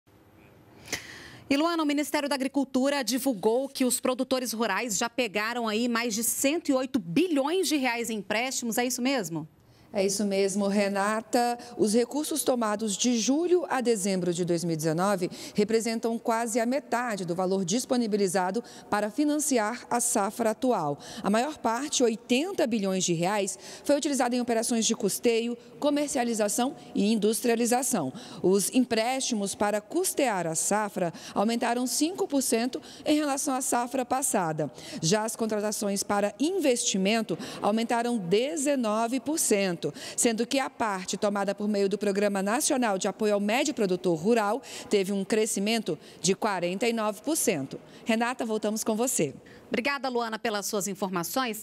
Crédito Rural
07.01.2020 - R$ 108 bilhões foram contratados por produtores rurais para financiar a safra 2019/ 2020.